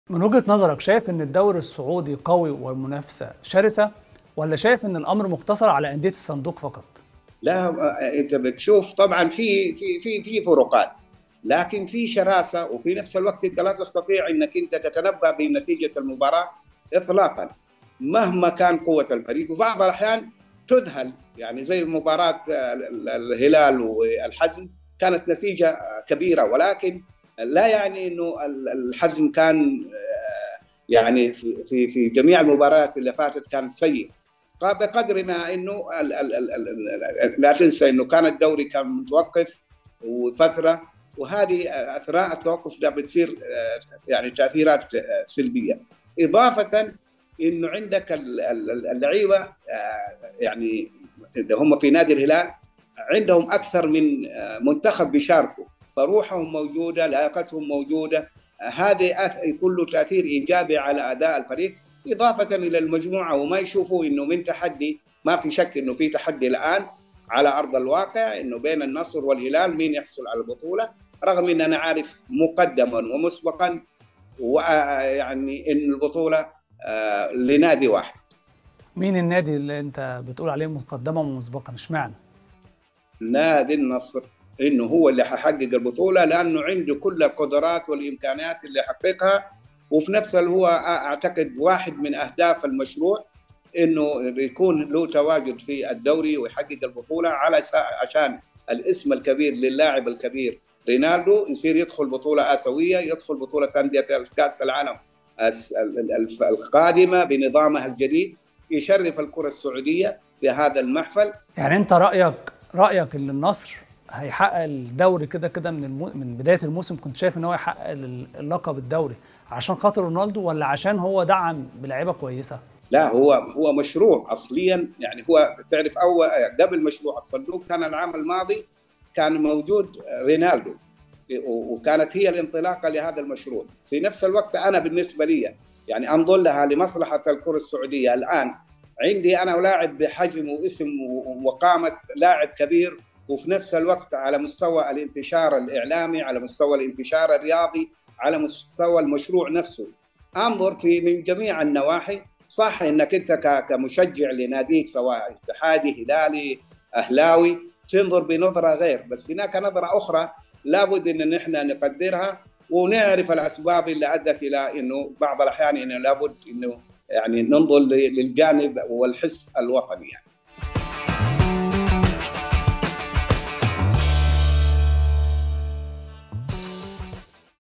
عدنان جستنيه الصحفي السعودي يكشف عن توقعه للفريق الفائز بالدوري السعودي
النصر، الهلال أم الاتحاد؟🤔